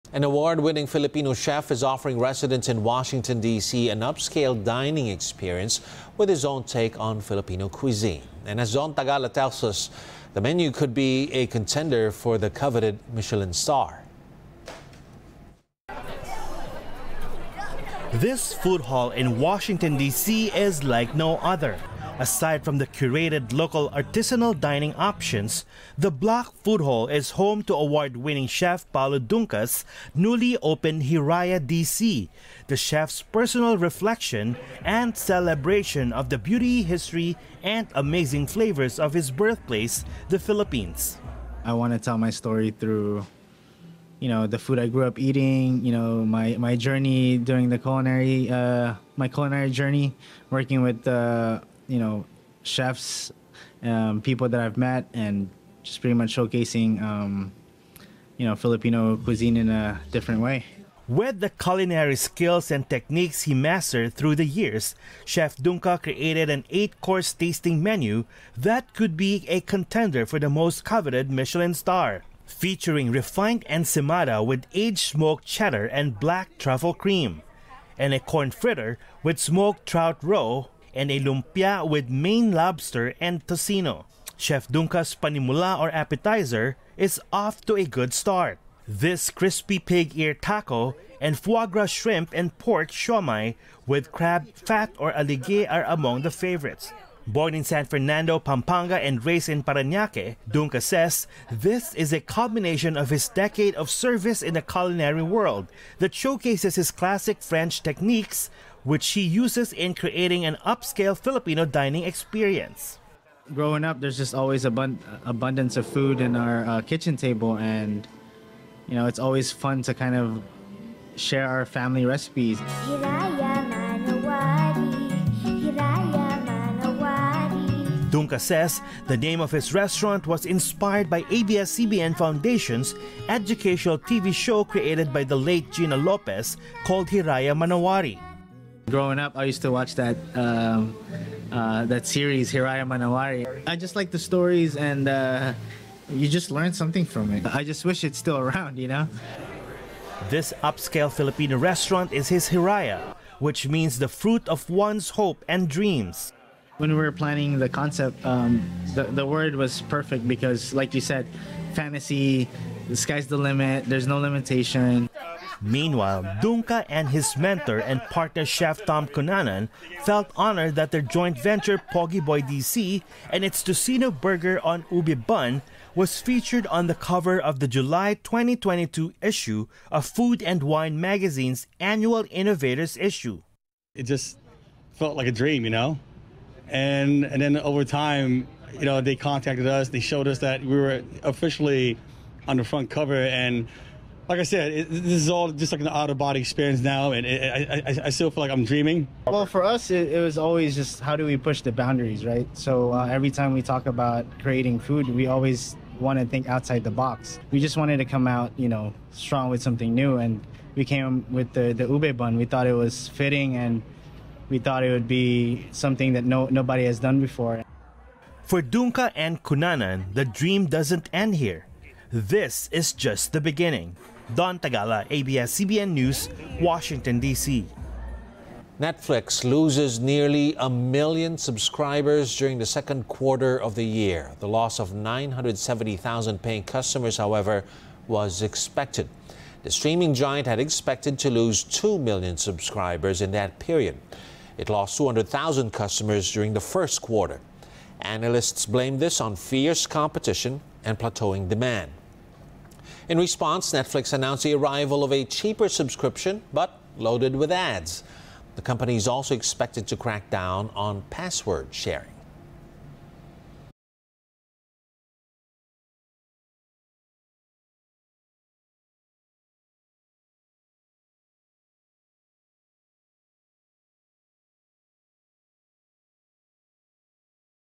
Award-winning chef showcases Filipino cuisine in Washington DC | ANC
Rundown: An award-winning Filipino chef is offering residents in Washington DC an upscale dining experience with his own take on Filipino cuisine.